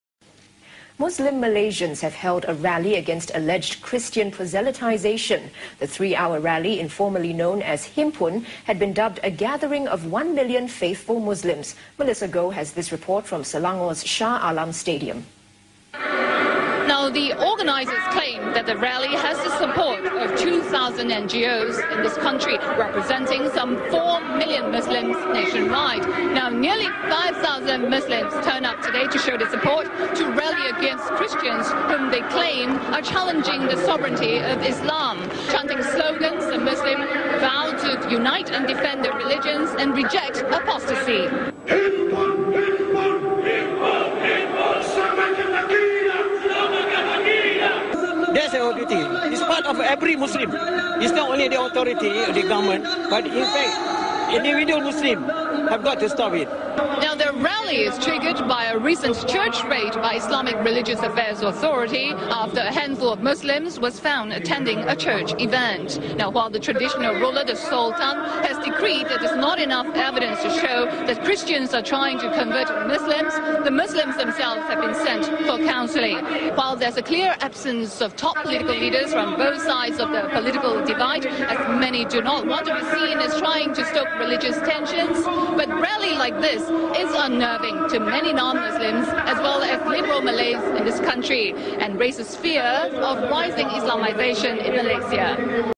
Muslims Rally Against Christians In Malaysia
Muslims in every country are losing their minds because every day thousands and thousands of Muslims are leaving Islam. Evil hates good and in Islam Muslims are slaves if any Muslim leaves Islam they must be killed, That is Mohammed's teaching and it is found in the quran and Muslims follow the quran.